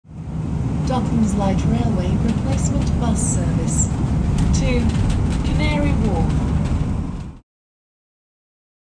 DLR to Canary Wharf
DLR Replacement Bus Service (3428) iBus announcement (18/06/2023)